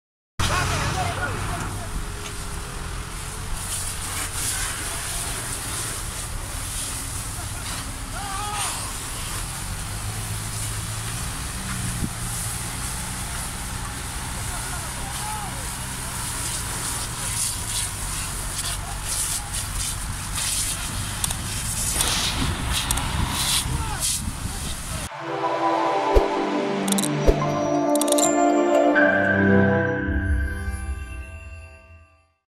Elektrik tellerine değen saman balyası yüklü TIR yandı
Mardin’in Kızıltepe ilçesinde, saman balyası yüklü TIR’ın elektrik tellerine değmesi sonucu çıkan kıvılcımların çıkardığı yangında, araç tamamen yandı.

Değerli İzleyicilerimiz;

TÜRKÇE

KÜRTÇE-KURMANCI

ARAPÇA

İNGİLİZCE

FARSÇA

KÜRTÇE-SORANİ